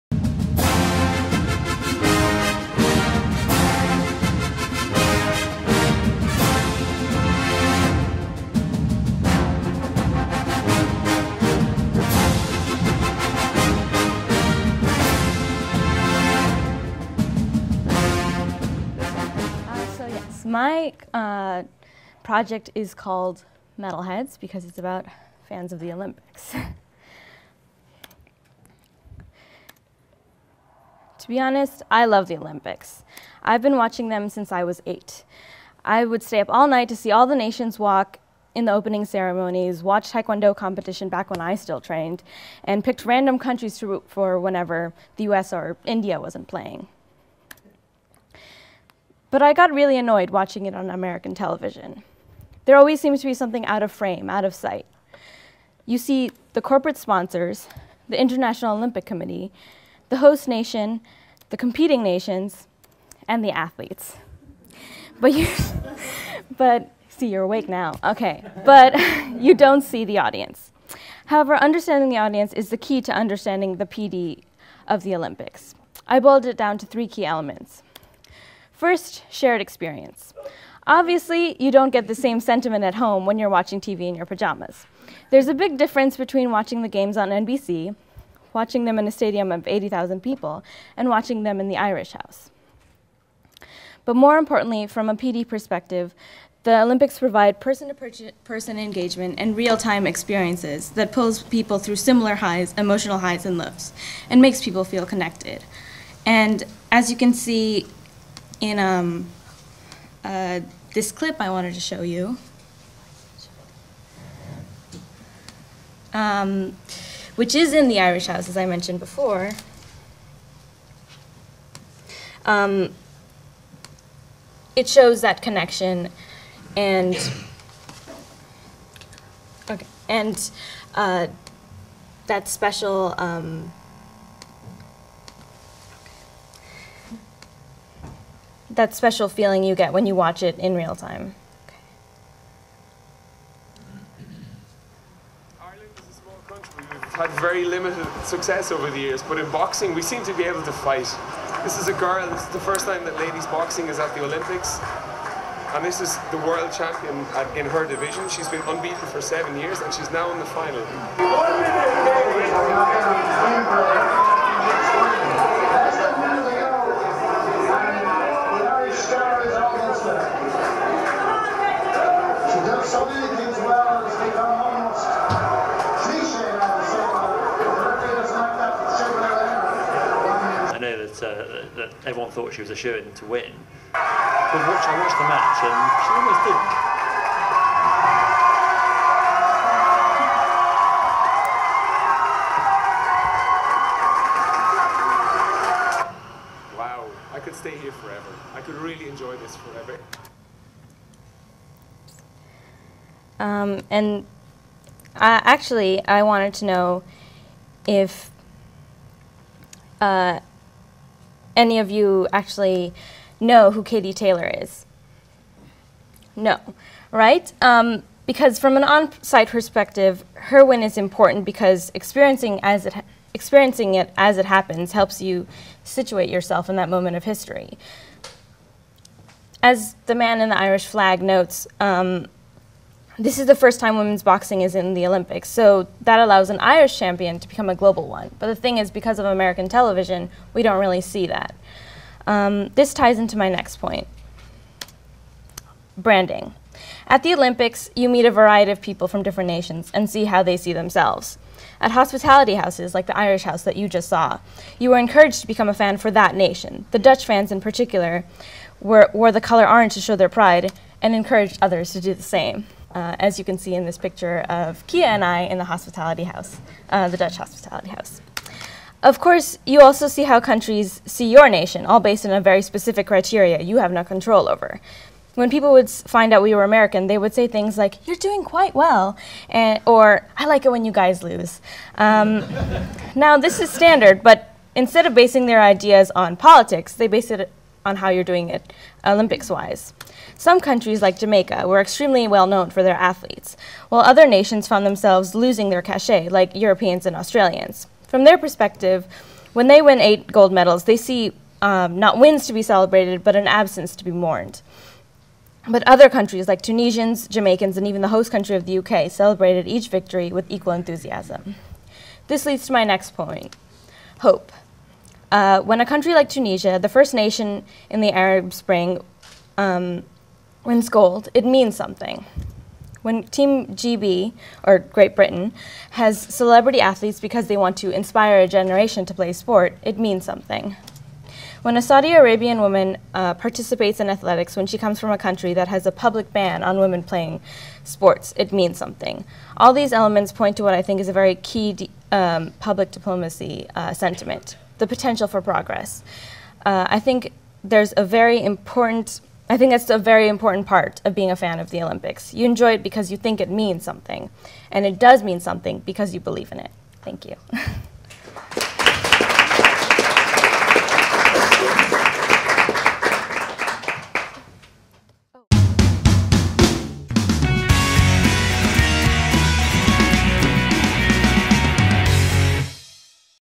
Sulagna Misra - 2013 Forum on Public Diplomacy at USC by APDS
Sulagna Misra presents her practicum on the Public Diplomacy of the Olympics. Based on her primary research at the London 2012 Olympics, Sulagna tells us why the Olympics matter truly matter